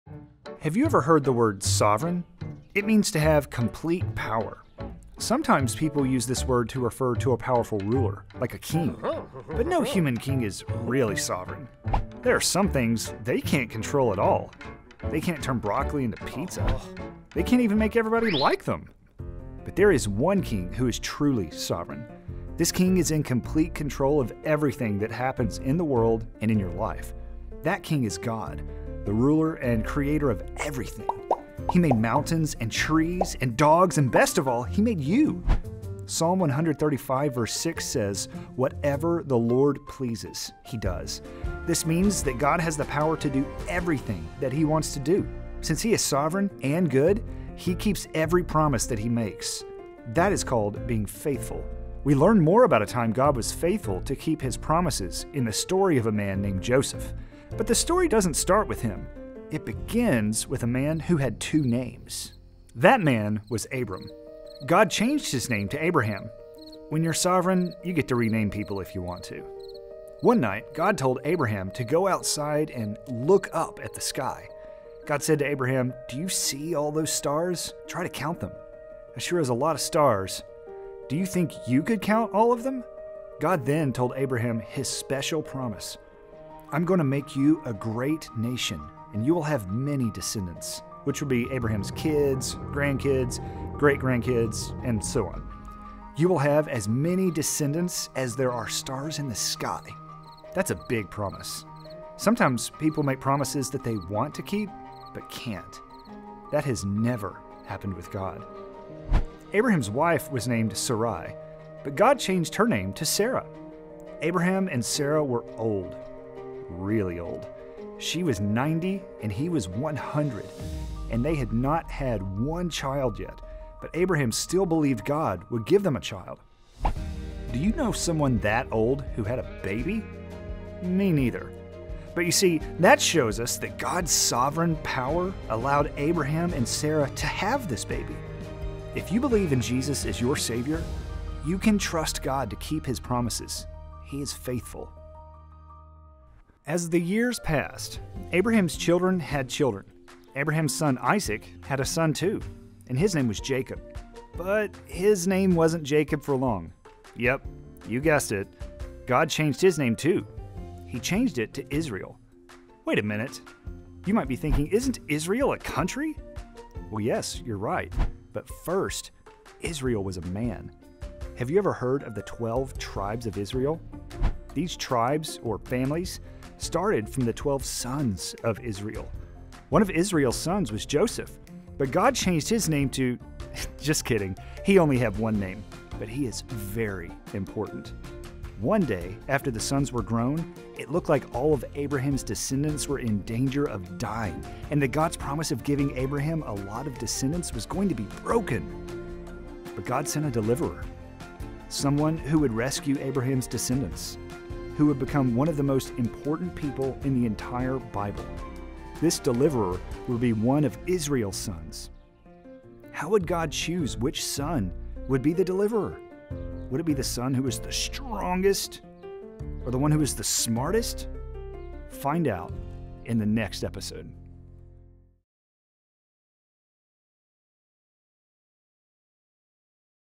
A Family Promise |Joseph: Dreams of Destiny Chapter 1
God made an exciting promise to begin a great nation. One of the most important characters in this story is Joseph who faithfully served God through many difficult circumstances.